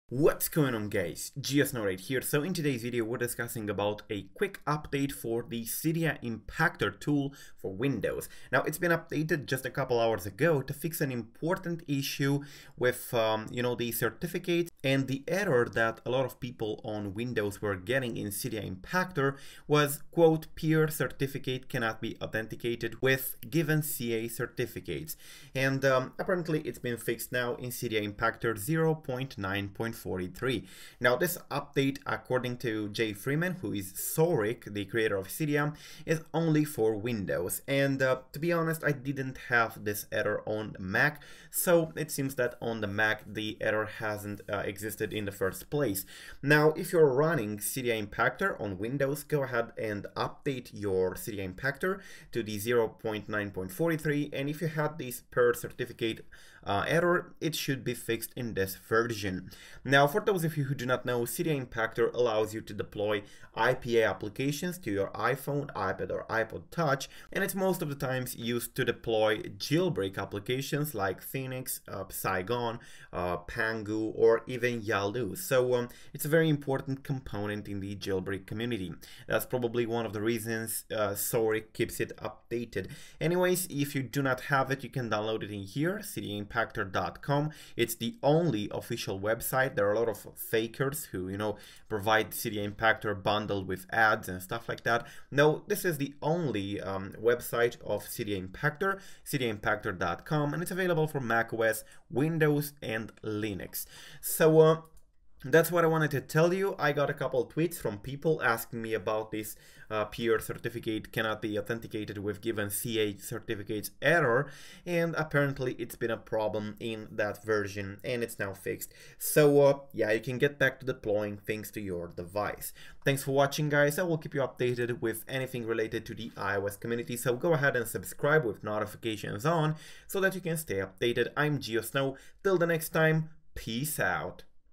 NEW Cydia Impactor UPDATE RELEASED for Windows | Peer Certificate Error Fixed!
saurik has updated just a few hours ago Cydia Impactor to version 0.9.43 which fixes an error on Windows, that would cause "Peer certificate cannot be authenticated with given CA certificates" to pop up for some users.

The macOS version seems to not be affected by the error, therefore, this update is Windows only. For those of you who don't know, Cydia Impactor is a tool developed by saurik, the developer of Cydia and it helps to deploy IPA (iOS Apps) to iDevices. It is primarily used for deploying jailbreak tools in IPA format, but any IPA can be installed with it.

By the way, since there are many websites providing Cydia Impactor on ad-infested pages, you should keep in mind that Cydia Impactor's official and only website is cydiaimpactor.com so please don't get Cydia Impactor from other sources as it might be an outdated version or even worse.